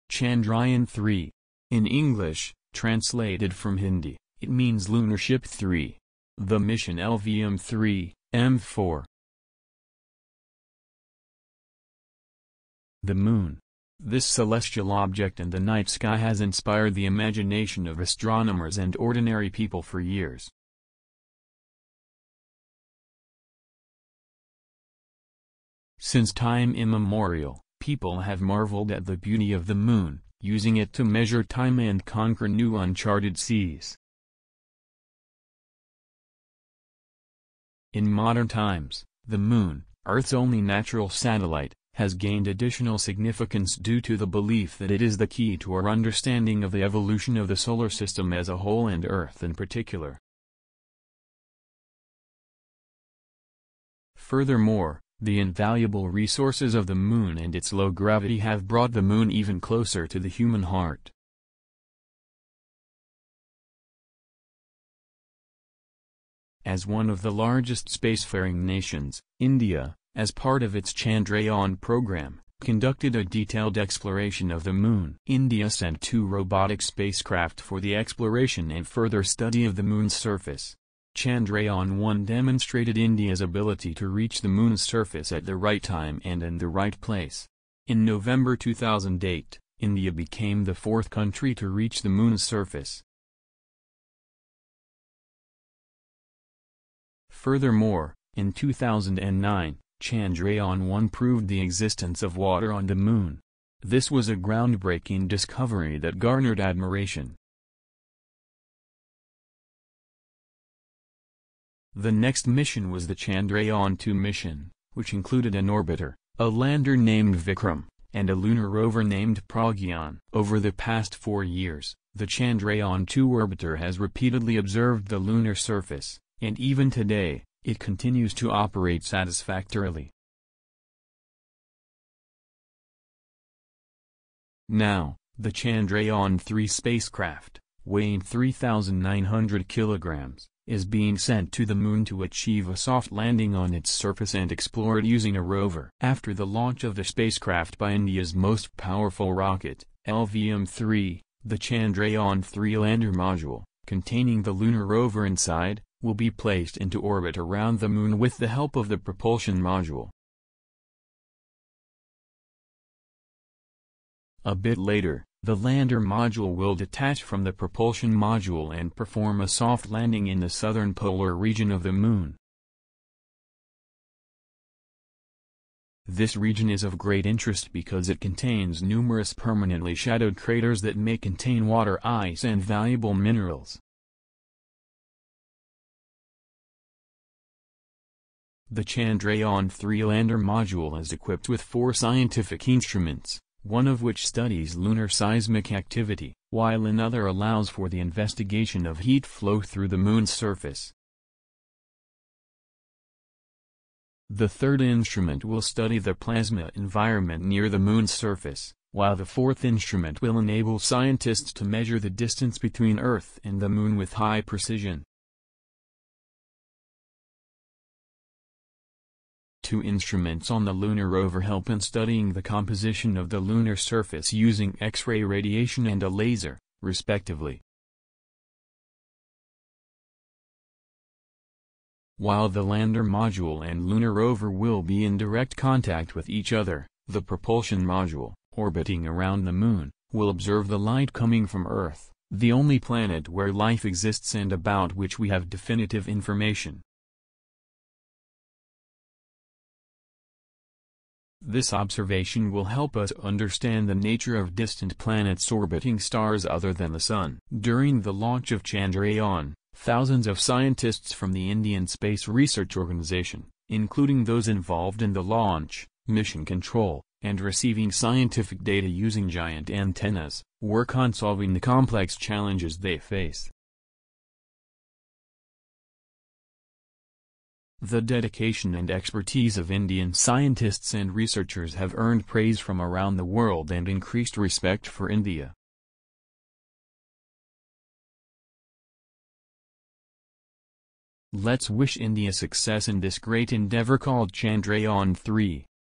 MOONSHIP 3 or CHANDRAYAAN 3
Welcome to the video that tells the story of the Indian lunar spacecraft Chandrayaan-3, which successfully completed its flight and soft landing on the Moon.